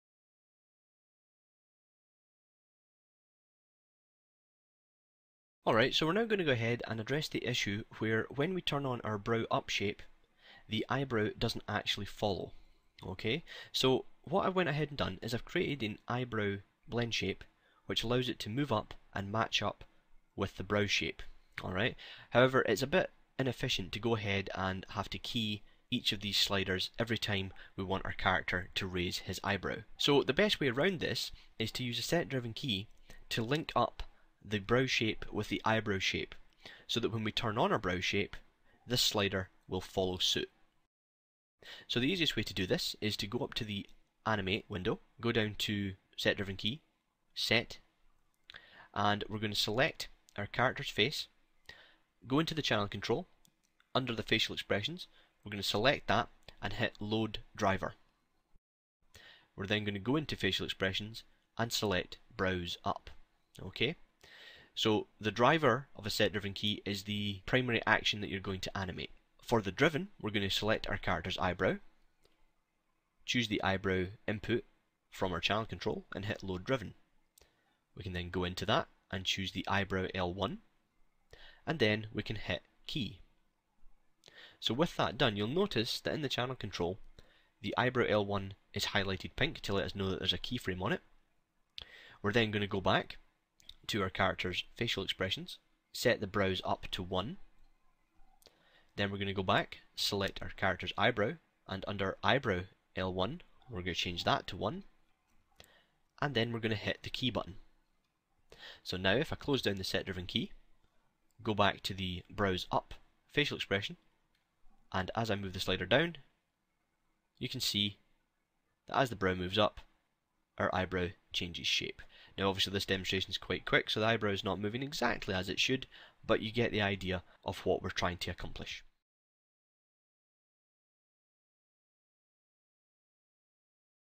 Maya tutorial: Control animation with blend shapes, Part 6
This video by Pat Imrie came with his tutorial in issue 168 of 3D World magazine.